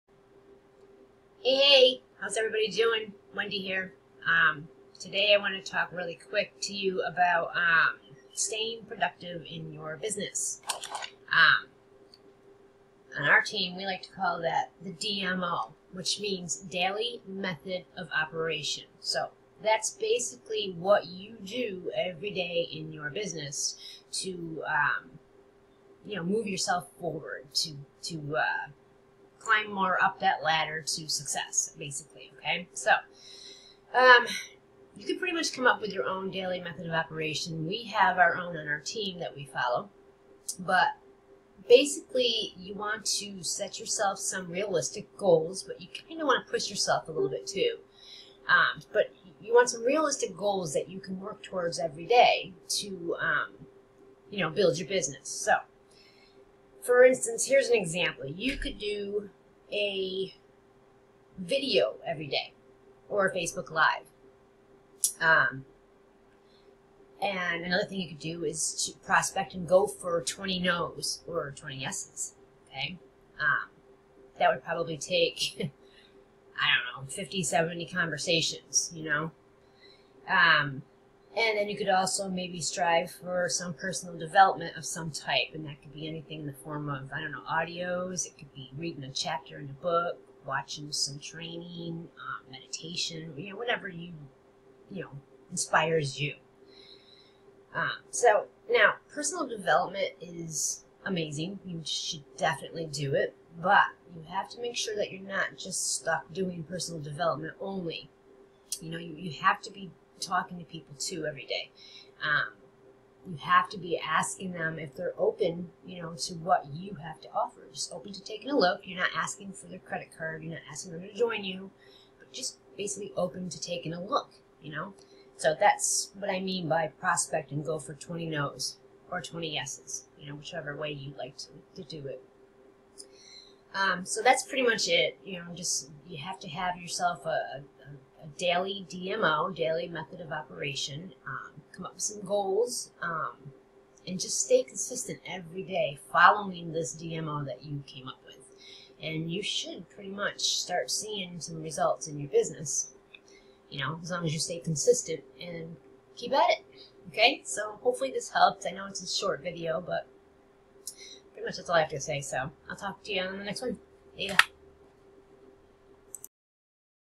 What's Your Daily DMO - Daily Method of Operation Strategy in your Network Marketing Business
What's your Daily Method of Operation?

What are you doing every day to build your network marketing business? Learn how to set your own effective DMO so you can start racing towards success.

Blessings!